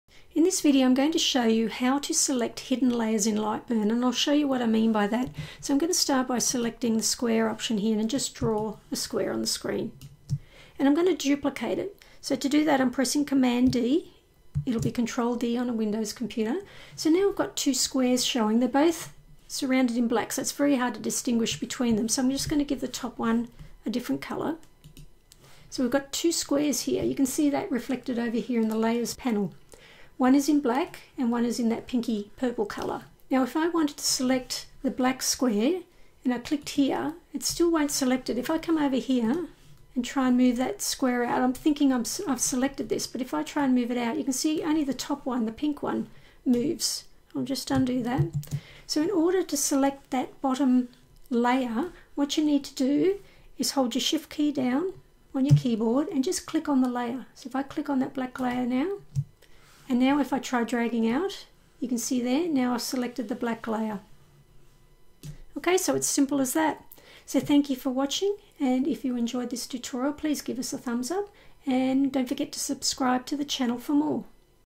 How to Select Hidden Shapes (Layers) in Lightburn
Learn how to select hidden shapes in Lightburn.